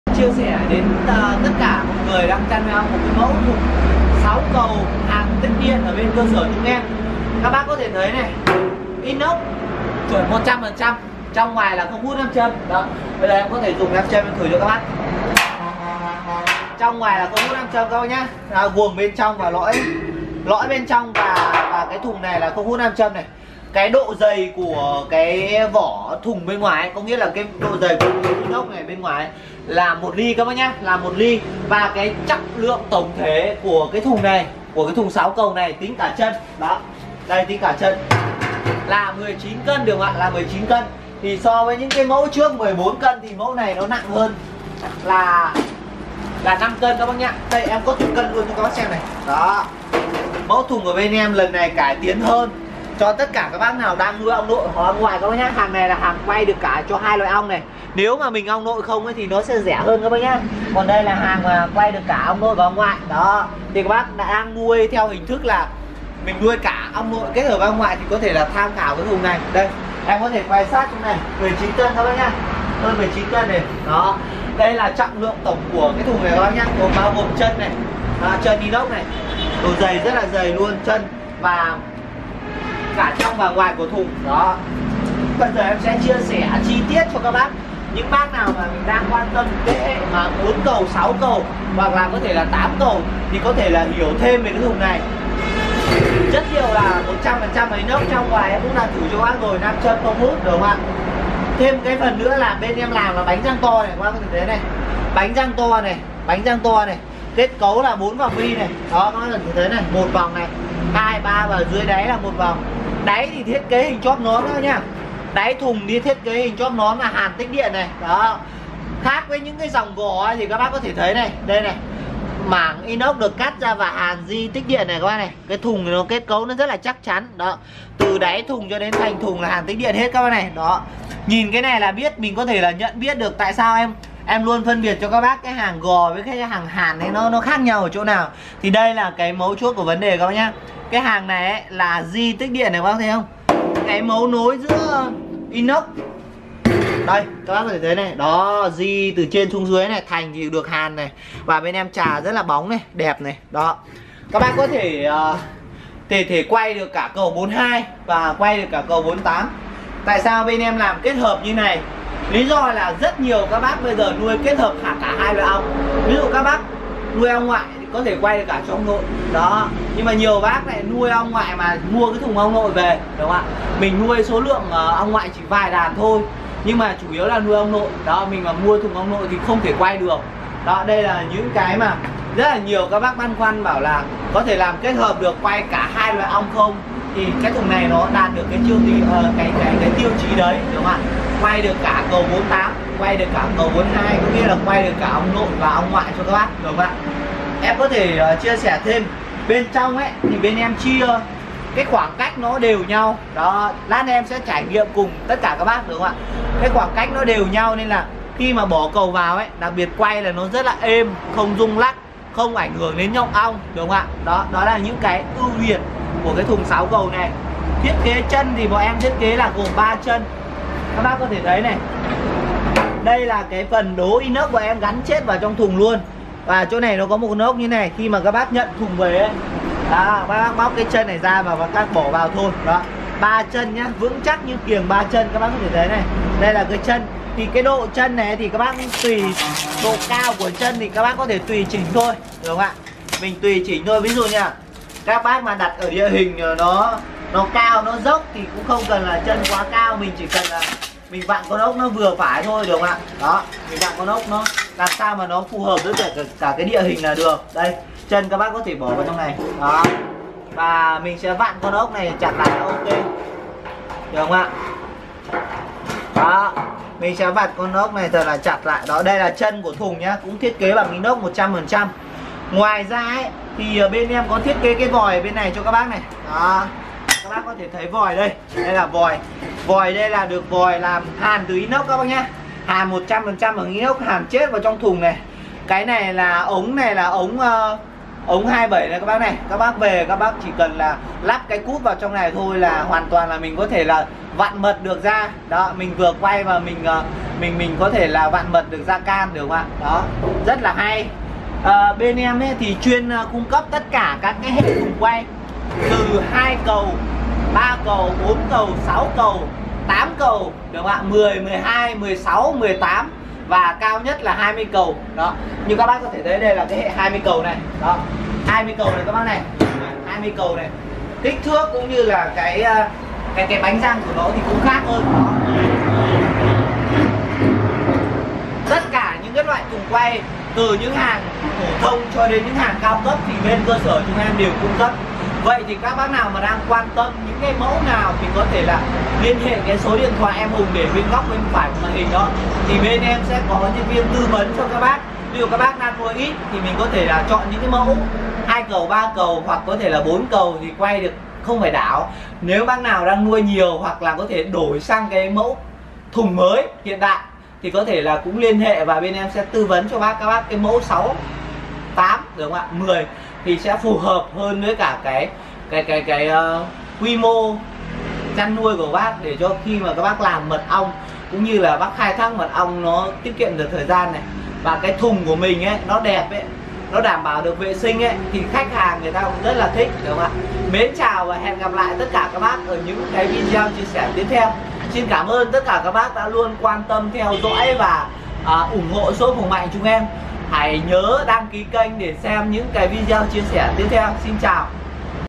Thùng quay mật 6 cầu inox cho cả Ong nội và Ong ngoại cao cấp
Thùng quay mật ong nội và ong ngoại 6 cầu inox không nhiễm không rỉ
Hàng chất lượng cao được sản xuất theo quy trình khép kín hiện đại mọi. Shop Hùng Mạnh  cơ sở cung cấp vật tư ngành nuôi ong mật phục vụ các bác nuôi ong.
- Ưu điểm thùng quay mật 6 cầu nhà Hùng Mạnh
+ 100% Inox không hút nam châm, không nhiễm, không sỉ sét, vệ sinh dễ dàng
+ Độ dày vỏ thùng 1li
+ Trọng lượng thùng 19kg, cho vòng quay đều không bị rung lắc
+ Dùng cho cả Ong nội và Ong ngoại
+ Bánh răng to, kết cấu 4 vòng bi
+ Đáy thùng thiết kế hình chóp nón, hàn tĩnh điện cao cấp
+ Mẫu mã đa dạng từ 2 cầu đến 6 cầu
+ Khoảng cách giữa các cầu đều nhau, không rung lắc, khi quay không ảnh hưởng đến nhộng ong
+ Thùng quay êm, lực quay nhẹ, tốc độ vừa đủ, không cần đảo cầu

Địa Chỉ : Tổ Dân Phố Bao Bì- Thị Xã Mỹ Hào- Mỹ hào - Hưng Yên